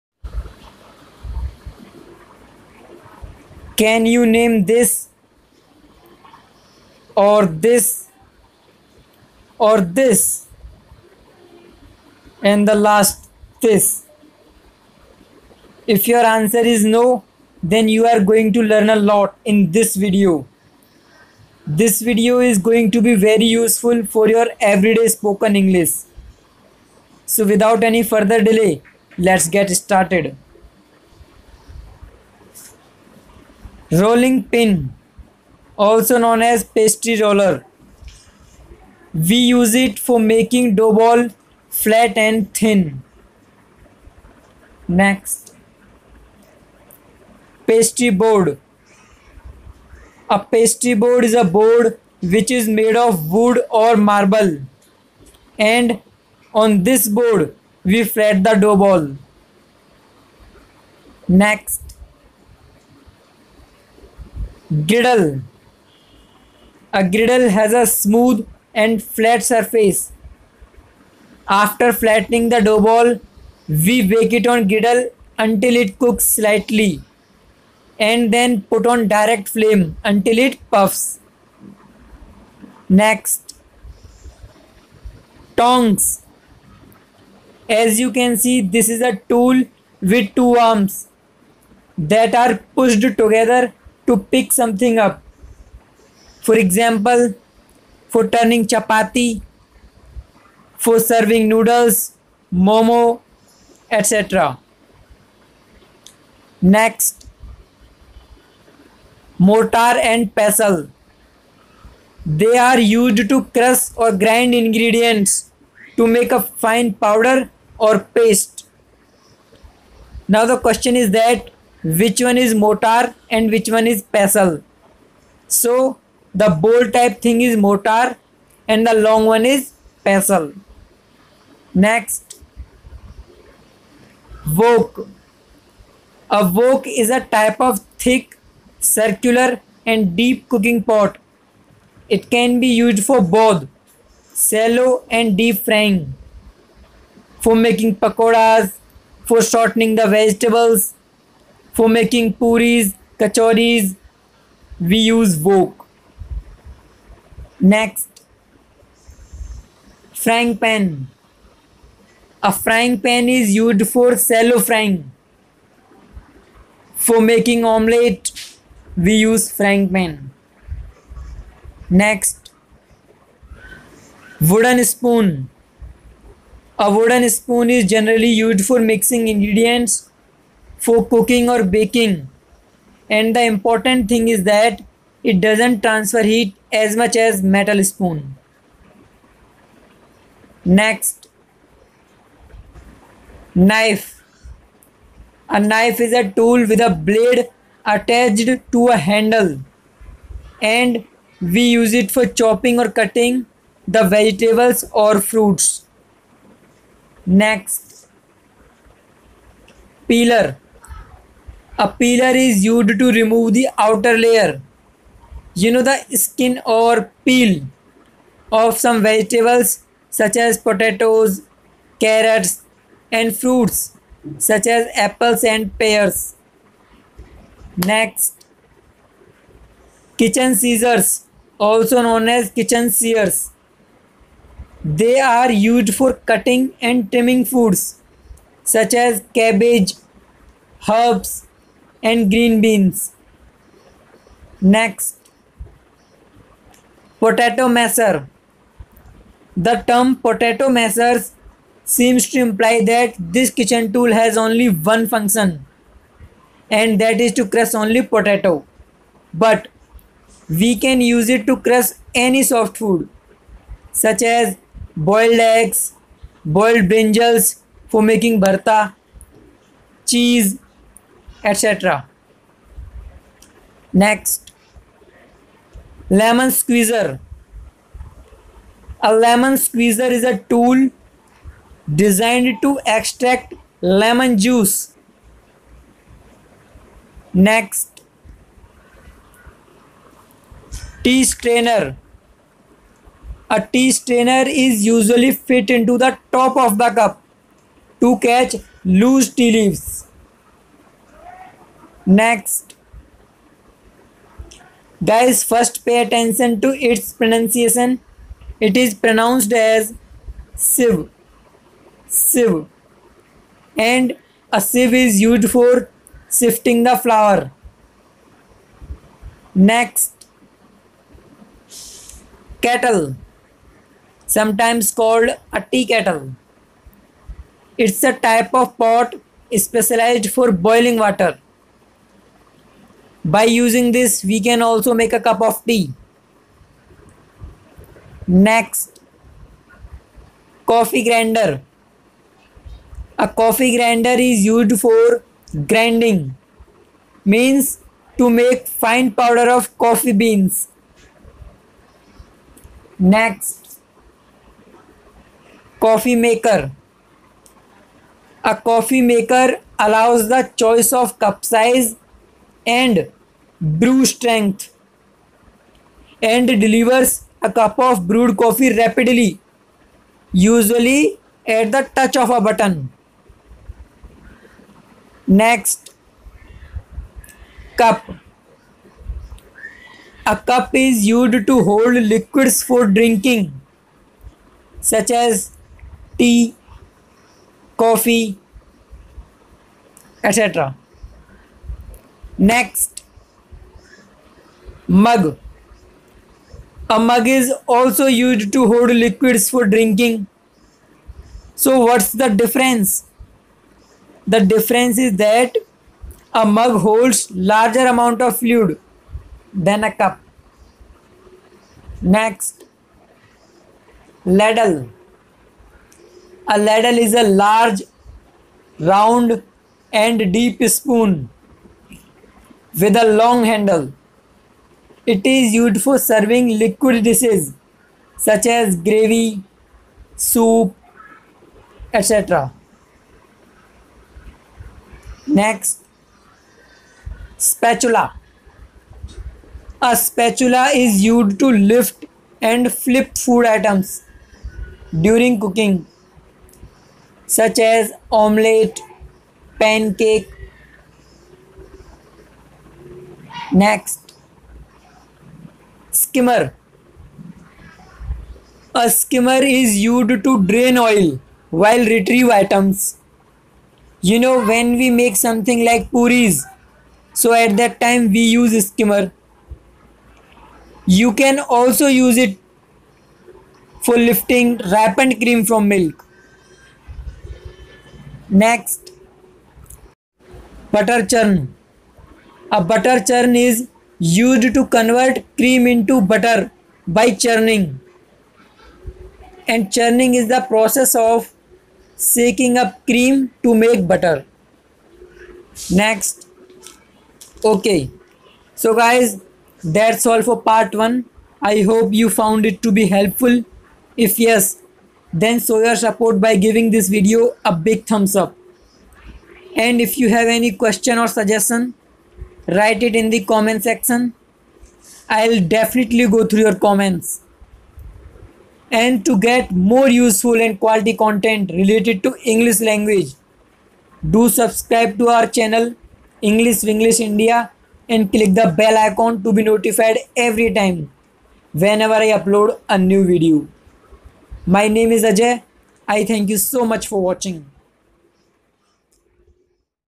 Kitchen Utensils || Kitchen Appliances || Daily Use Kitchen Vocabulary || English with Ajayraj
Take your spoken English game to the next level with English with Ajayraj

In this video, I have covered the following things:

1. Kitchen tools and appliances
2. Coking words
3. Kitchen vocabulary
4. Kitchen tools
5. Kitchen Utensils
6. Kitchen appliances